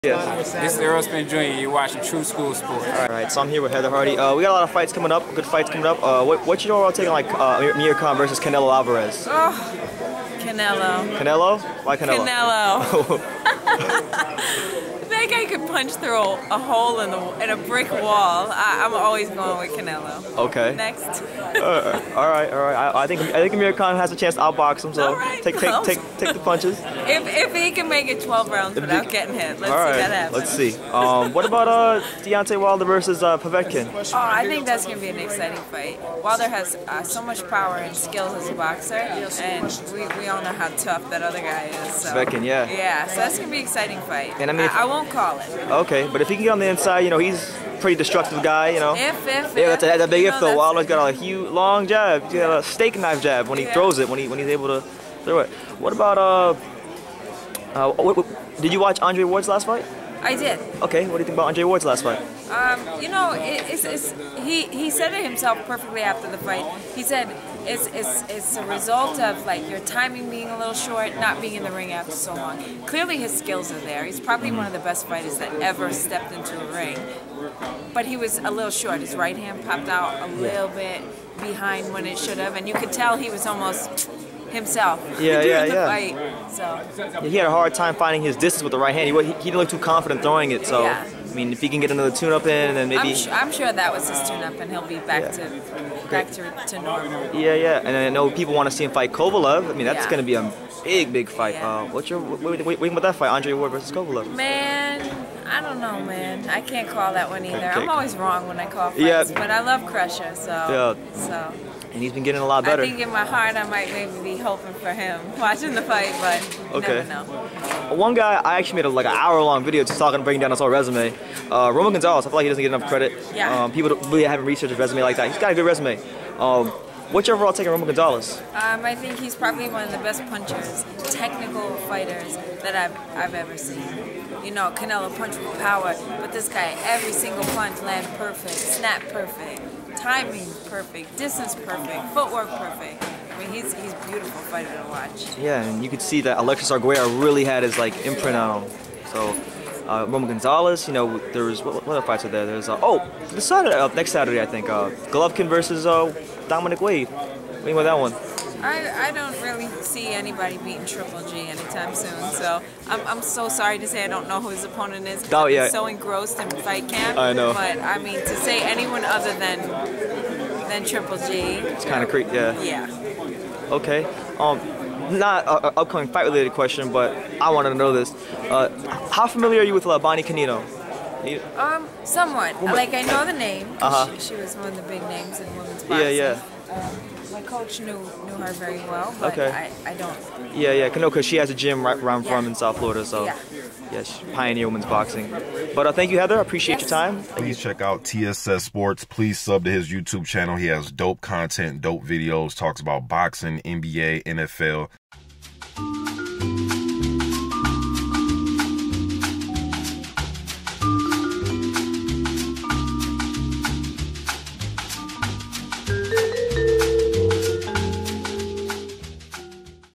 Heather Hardy breaks down Canelo, Andre Ward & Roman Gonzalez
Heather Hardy gives her thoughts on upcoming fights